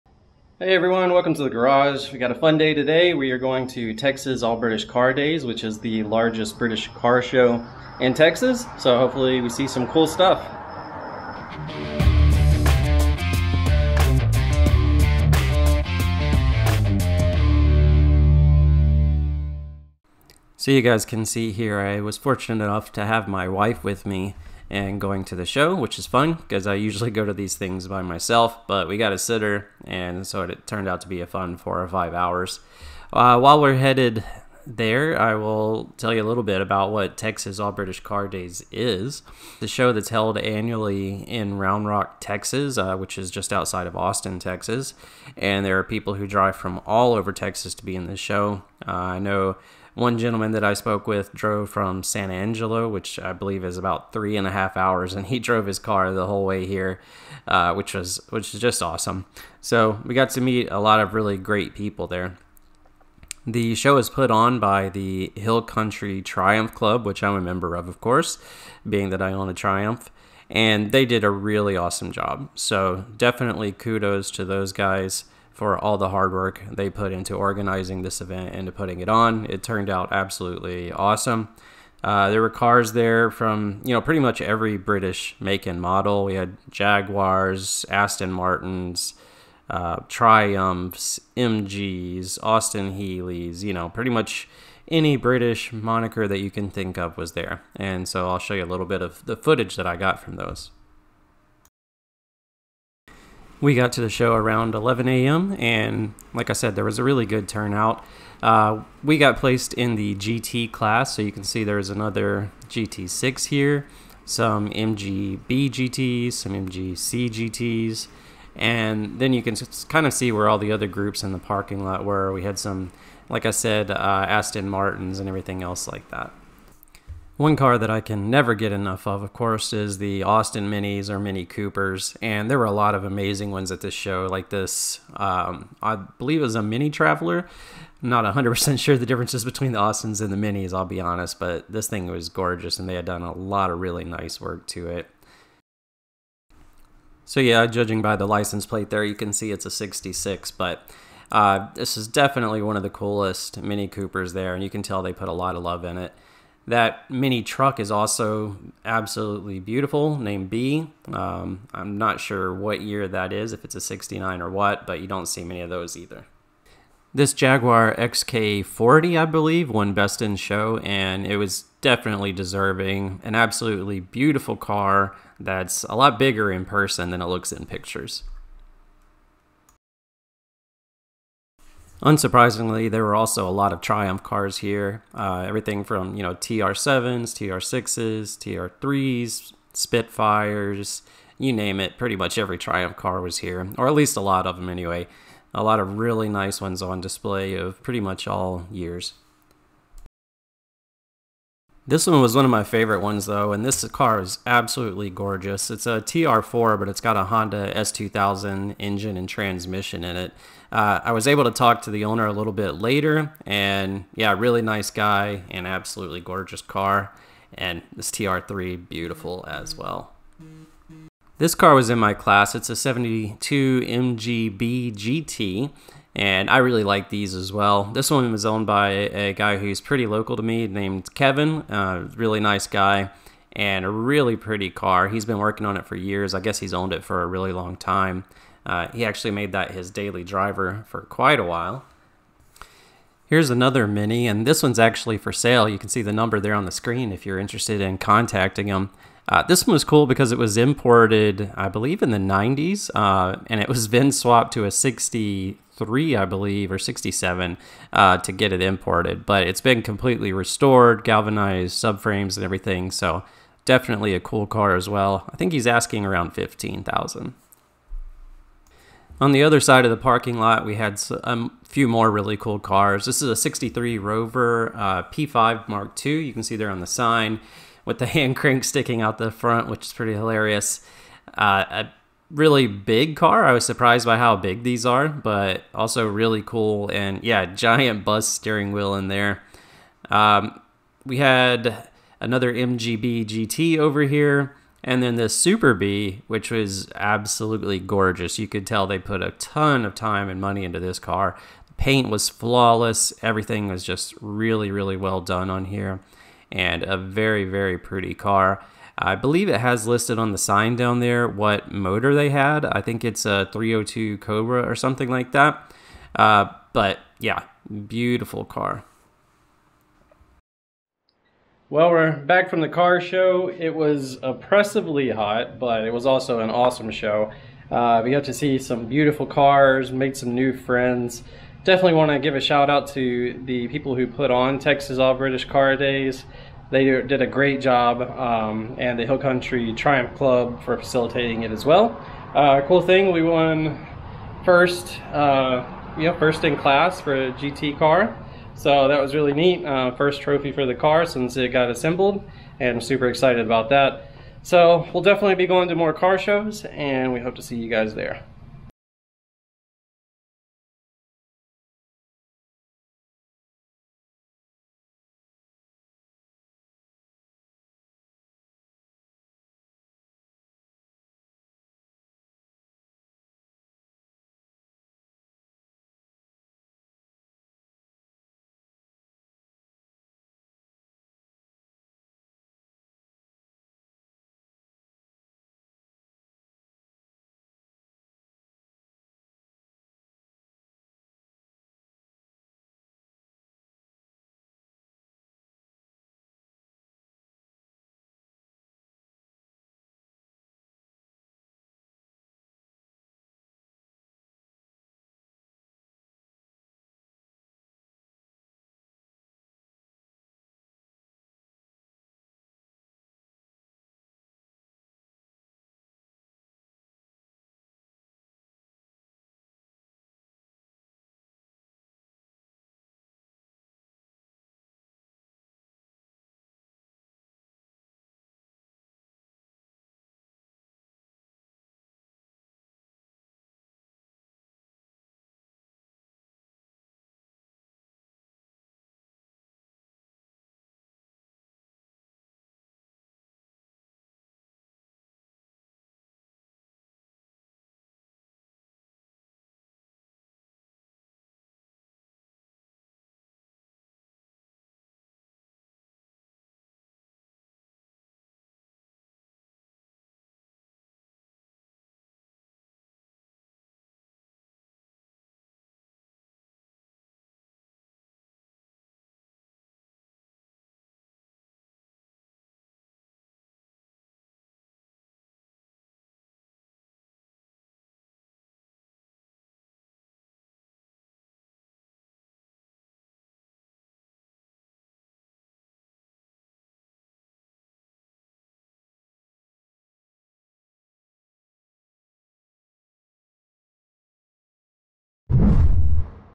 GT6 - Texas All British Car Days
We went to an awesome British car show in Round Rock Texas!